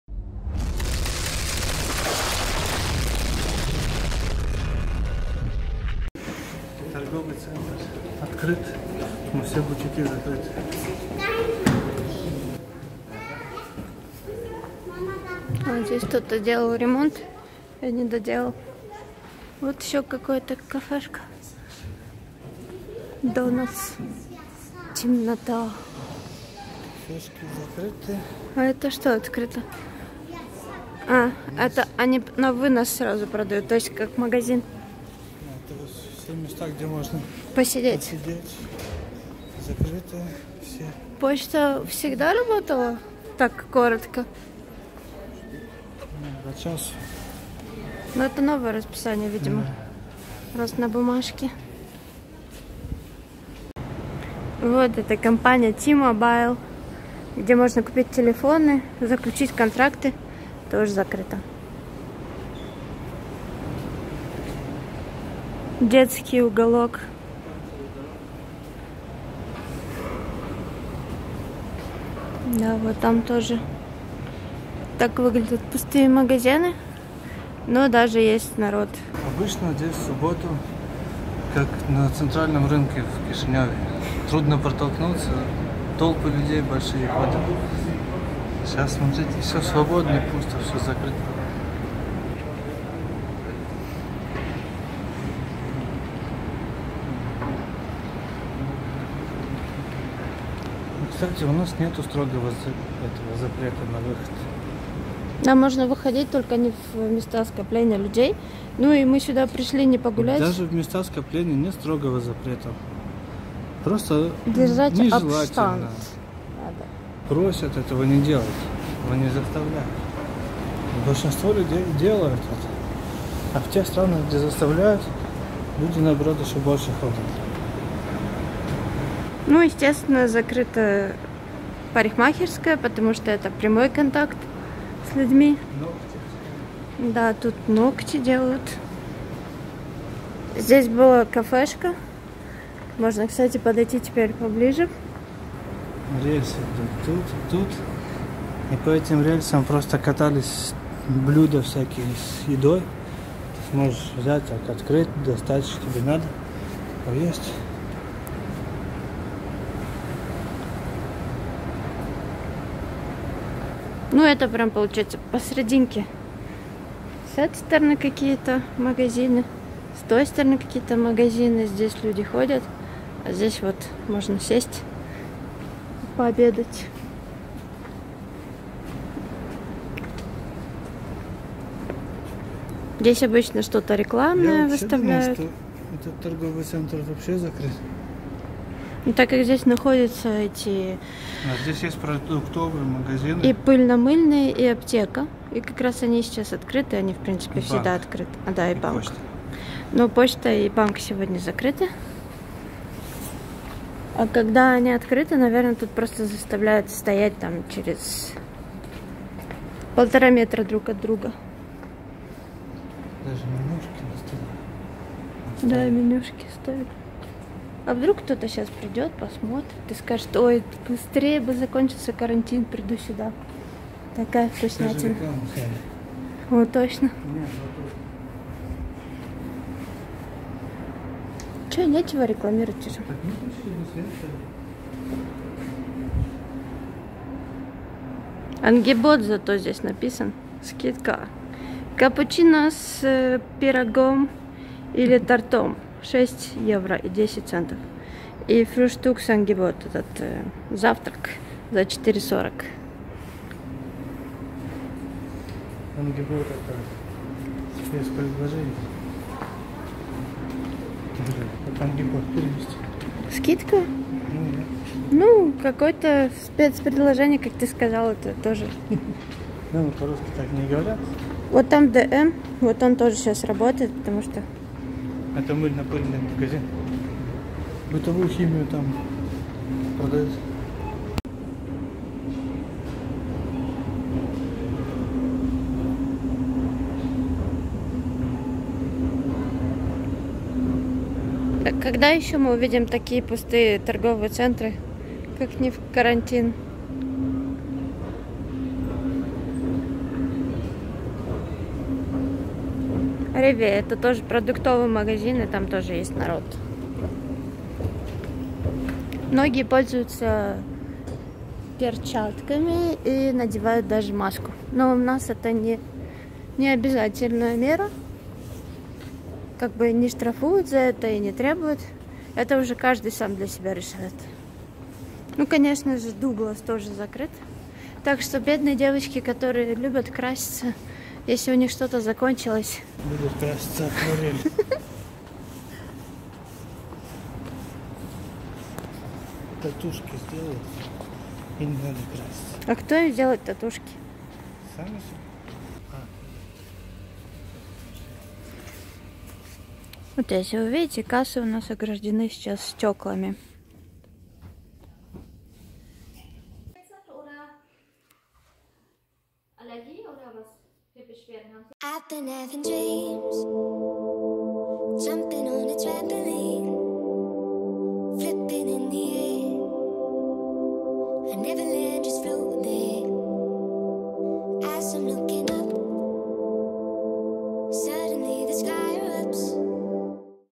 #221 Пустые торговые центры во время карантина в Германии
Это очень мотивирует создавать новые ролики.